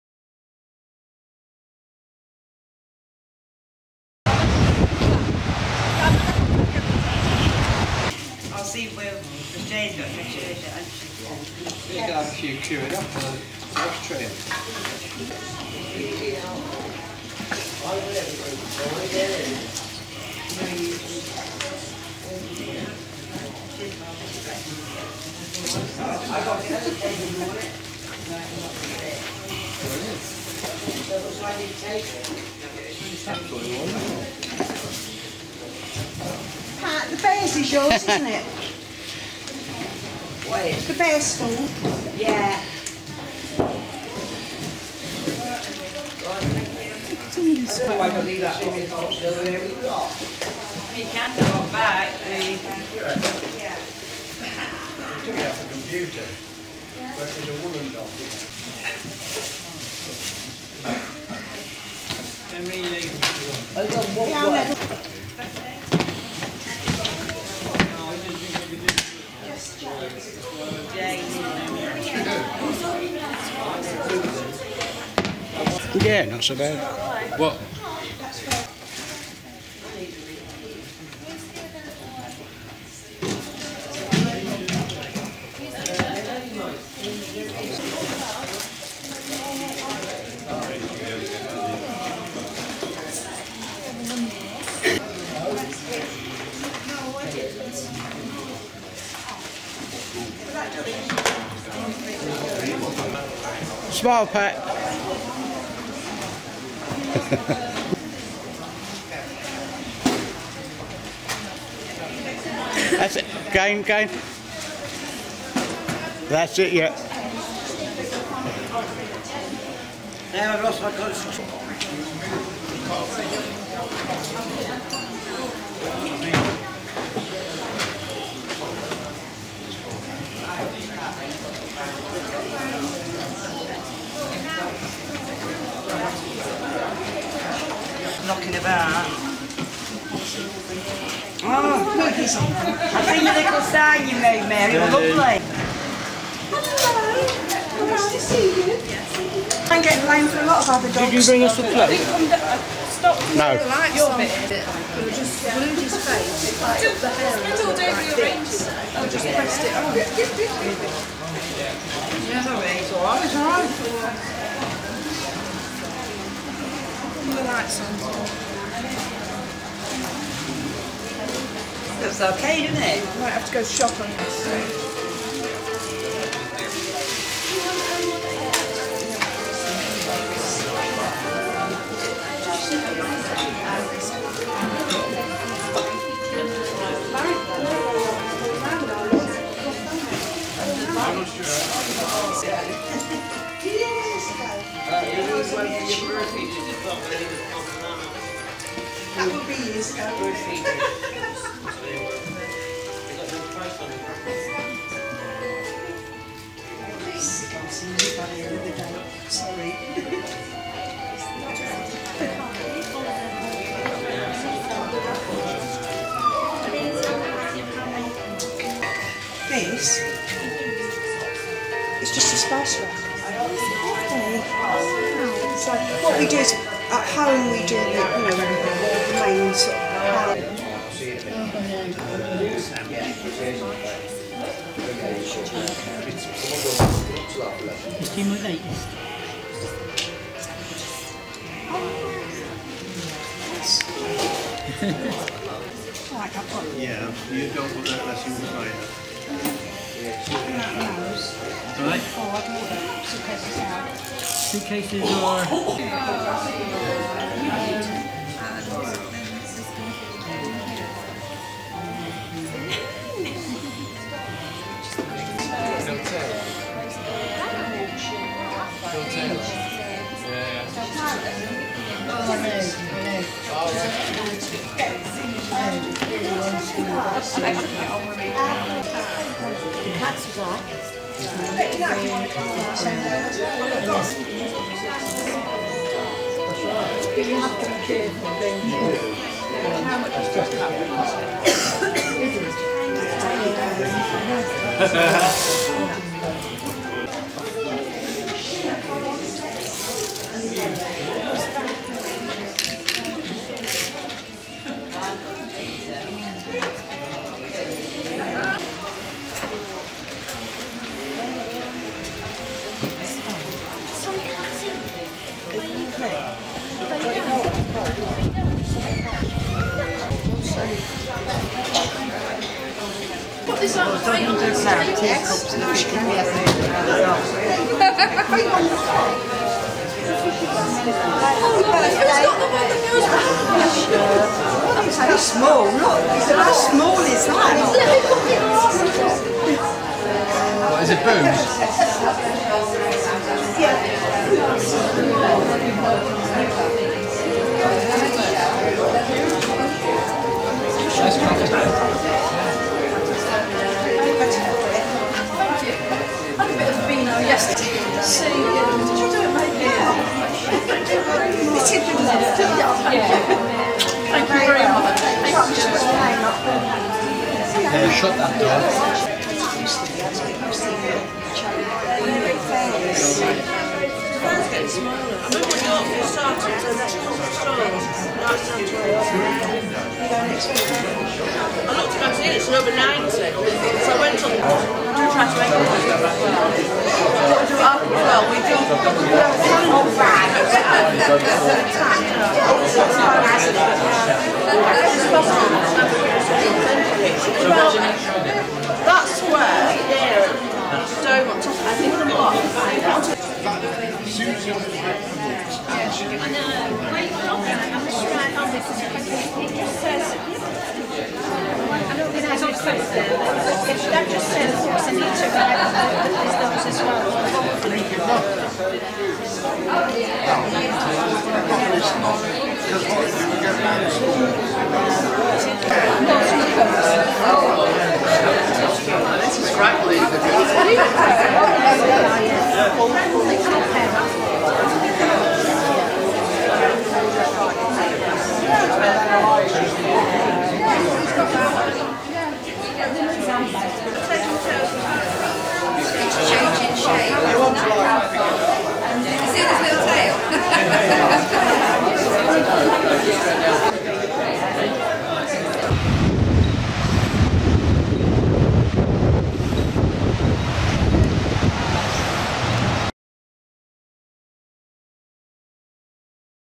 the little mermaids dollshse club mablethorpe 29 mAY 2016 CRAFT FAIR AND EXHIBITION
This is THE LITTLE MERMAIDS DOLLSHOUSE CLUB  and Craft Fair exhibition on the 29th May 2016